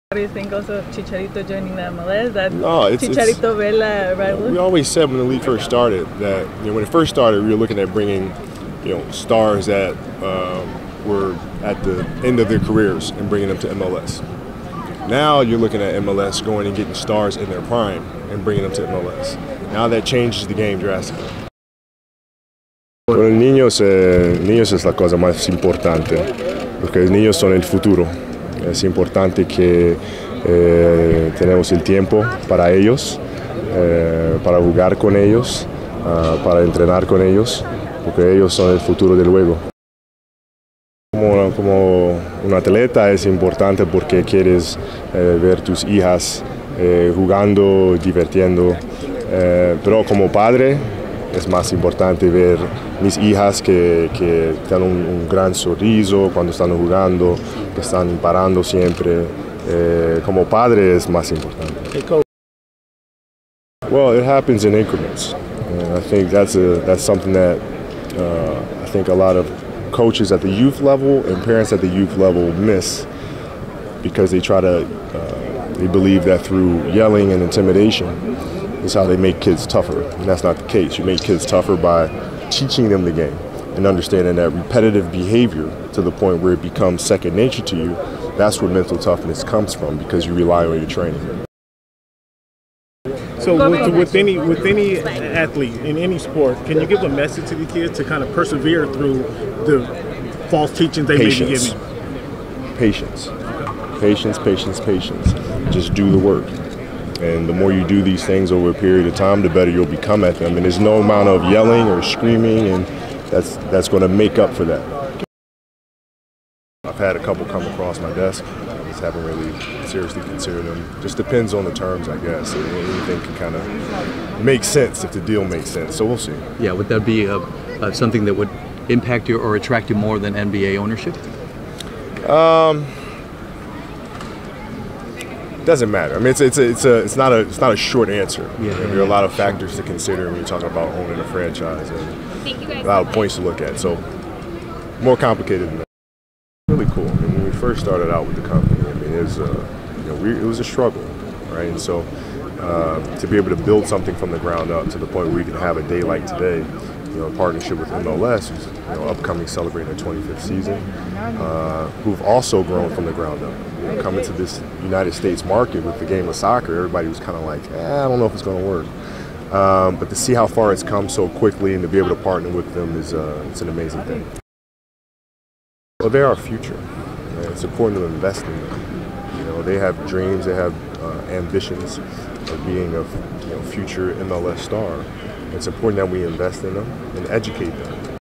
MLS x Body Armor Partnership
Kobe Bryant and Body Armor announce their partnership with Major League Soccer for their 25 year Anniversary